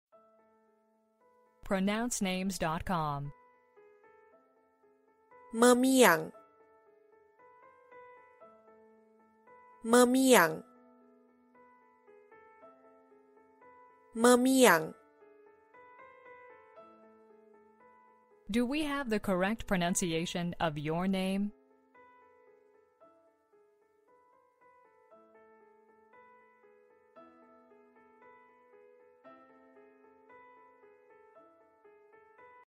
How do you say Memiang in Malaysia? Malaysian pronunciation of Memiang - Pronounce Names
How do you pronounce Memiang in Malaysia? For more information about Memiang, such as gender, origin, etc.,